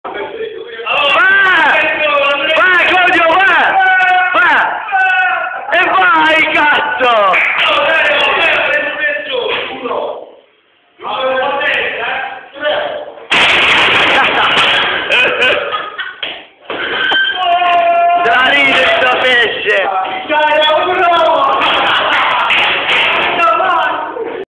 Come fare strike con una sedia
Il Re ci dimostra come fare strike con una semplice sedia.